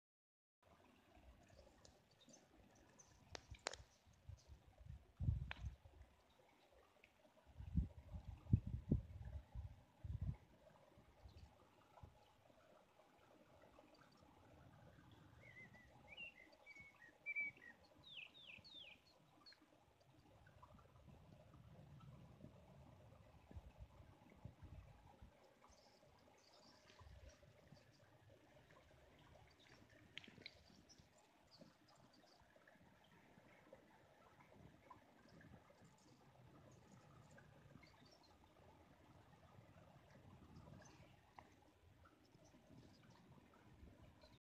Outdoors - Stream - Frog likes being Filmed
Stream  - Frog  - Enjoying  Outdoors